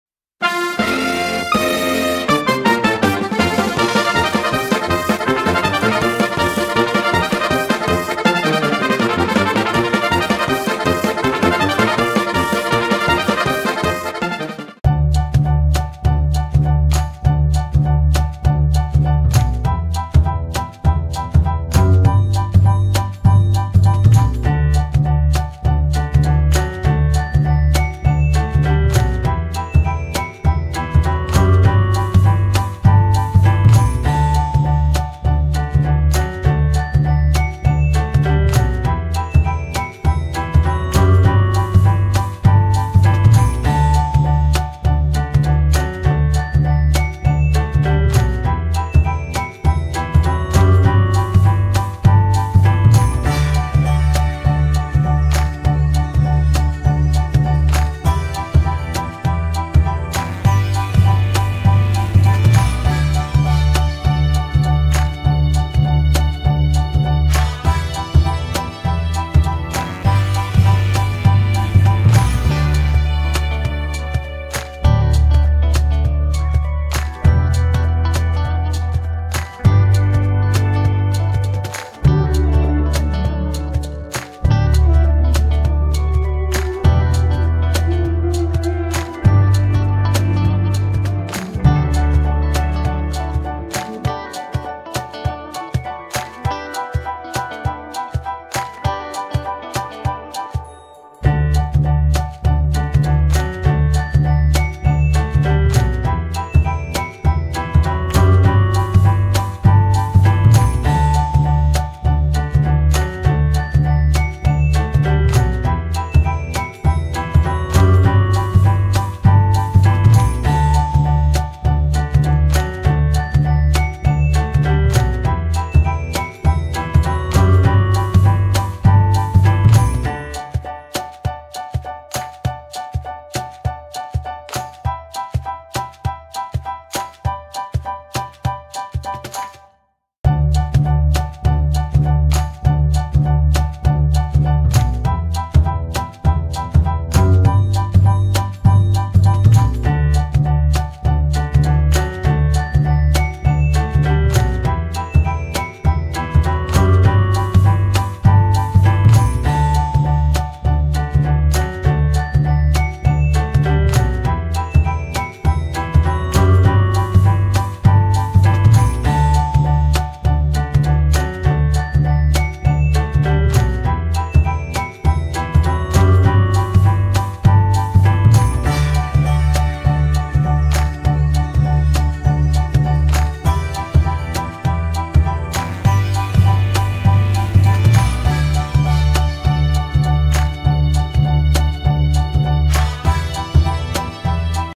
How to draw cute lion step by step | Easy cute lion Drawing for kids
Hello Welcome in this easy-to-follow drawing tutorial, we'll show you how to draw Tacos step by step and color it